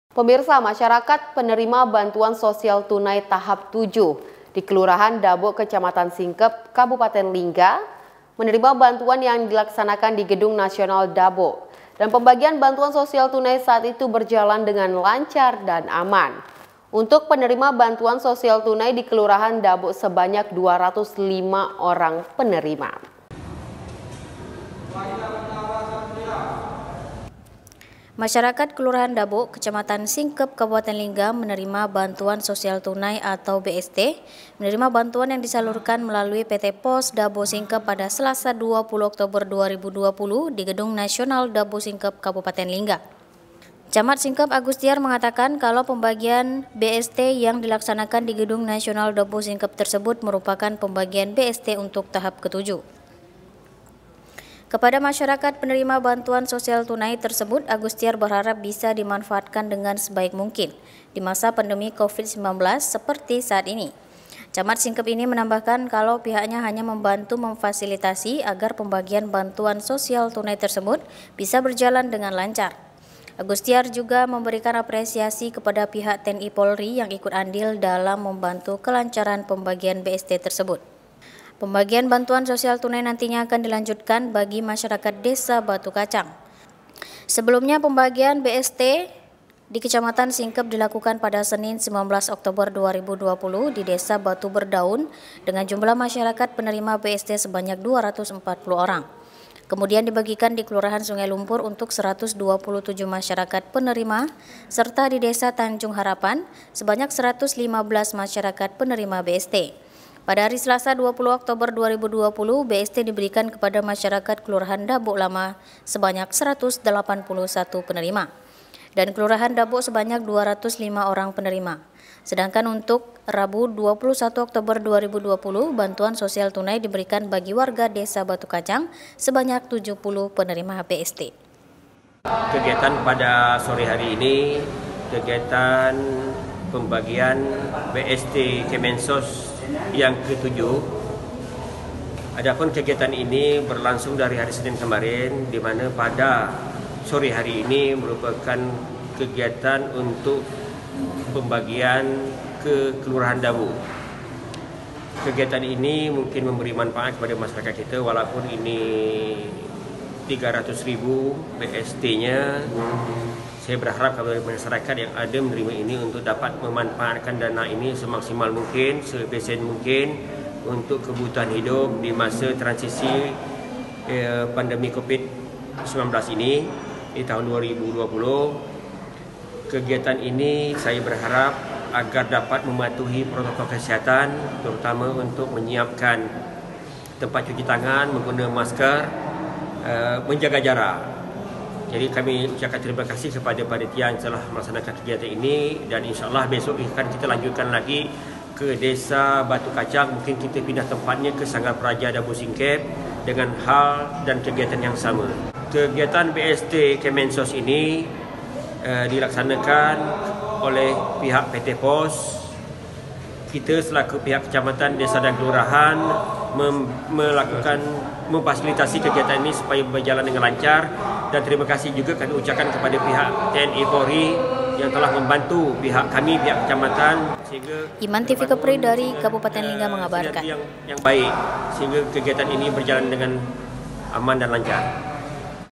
PEMBAYARAN BST TAHAP 7 DI KECAMATAN SINGKEP - TV KEPRI
//MASYARAKAT KELURAHAN DABO KECAMATAN SINGKEP KABUPATEN LINGGA PENERIMA BANTUAN SOSIAL TUNAI ATAU BST MENERIMA BANTUAN YANG DISALURKAN MELALUI PT POS DABO SINGKEP PADA SELASA (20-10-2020) DI GEDUNG NASIONAL DABO SINGKEP KABUPATEN LINGGACAMAT SINGKEP AGUSTIAR MENGATAKAN KALAU PEMBAGIAN BANTUAN SOSIAL TUNAI YANG DILAKSANAKAN DI GEDUNG NASIONAL DABO SINGKEP TERSEBUT MERUPAKAN PEMBAGIAN BST UNTUK TAHAP KE TUJUHKEPADA MASYARAKAT PENERIMA BANTUAN SOSIAL TUNAI TERSEBUT AGUSTIAR BERHARAP BISA DIMANFAATKAN DENGAN SEBAIK MUNGKIN DI MASA PANDEMIK COVID SEMBILAN BELAS SEPERTI SAAT INI CAMAT SINGKEP INI MENAMBAHKAN KALAU PIHAKNYA HANYA MEMBANTU MEMPASILITASI AGAR PEMBAGIAN BANTUAN SOSISL TUNAI TERSEBUT BISA BERJALAN DENGAN LANCARAGUSTIAR JUGA MEMBERIKAN APRESIASI KEPADA PIHAK TNI-POLRI YANG IKUT ANDIL DALAM MEMBANTU KELANCARAN PEMBAGIAN BST TERSEBUTPEMBAGIAN BANTUAN SOSIAL TUNAI NANTINYA AKAN DILANJUTKAN BAGI MASYARSKAT DESA BATU KACANGSEBELUMNYA PEMBAGIAN BANTUAN SOSIAL TUNAI DI KECAMATAN SINGKEP DILAKUKAN PADA SENIN (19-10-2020) TADI DI DESA BATU BERDAUN DENGAN JUMLAH MASYARAKAT PENERIMA BST SEBANYAK DUARATUS EMPATPULUH ORANGKEMUDIAN DIBAGIKAN DI KELURAHAN SUNGAI LUMPUR UNTUK SERATUS DUAPULUH TUJUH MASYARAKAT PENERIMA SERTA DI DESA TANJUNG HARAPAN SEBANYAK SERATUS LIMABELAS MASYARAKAT PENERIMA BSTPADA HARI SELASA (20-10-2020) BANTUAN SOSIAL TUNAI DIBERIKAN KEPADA MASYARAKAT KELURAHAN DABO LAMA SEBANYAK SERATUS DELAPAN PULUH SATU PENERIMA DAN KELURAHAN DABO SEBANYAK DUARATUS LIMA ORANG PENERIMA SEDANGKAN UNTUK RABU (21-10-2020) BANTUAN SOSIAL TUNAI DIBERIKAN BAGI WARGA DESA BATU KACANG SEBANYAK TUJUHPULUH PENERIMA BST.

KABAR KEPRI / RABU , 21 OKTOBER 2020